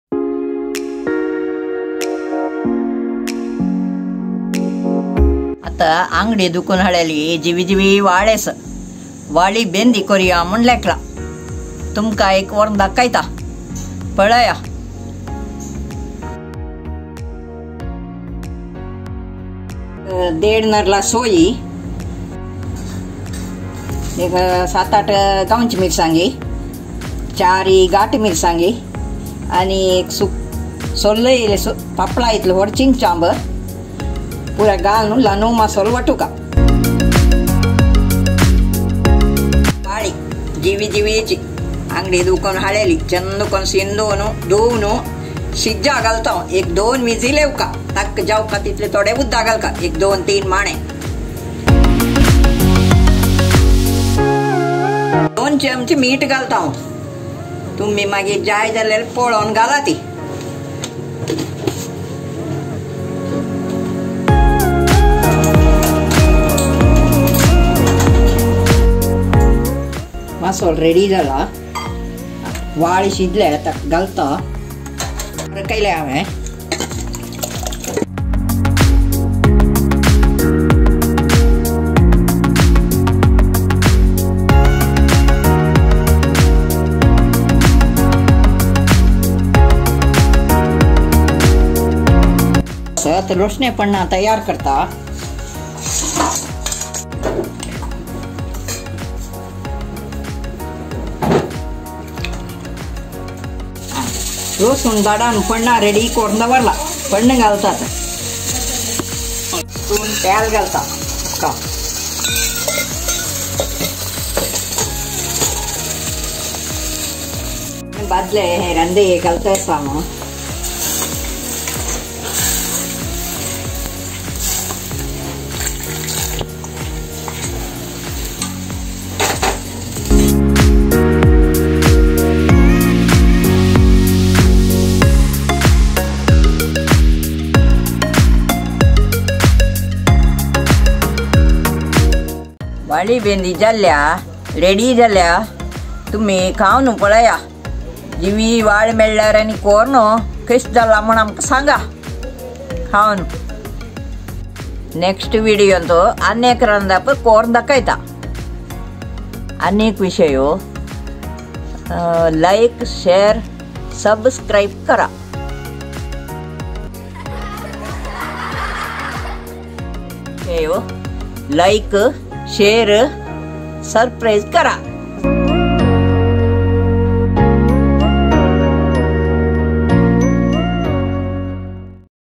Granny's Recipe - Vaali Bendi/Malabar Spinach Bendi
Malabar Spinach is the juicy yet tasty spinach which grows in southern west coast, India. The Malabar Spinach is called as Vali in Konkani. The curry prepared in the video is a GSB Konkani special. The name of the curry is "Vali Bendi".
The Curry tastes yummy. Please do try at home and feedback..